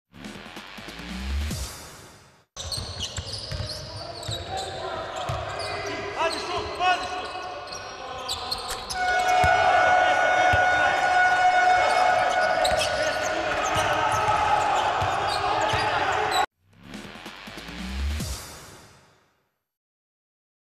Jonathan Davis scores from downtown (Borac - Zlatibor, 16.11.2020)
Jonathan Davis of Borac has come up with this long-range three-pointer in the game against Zlatibor. Check it out!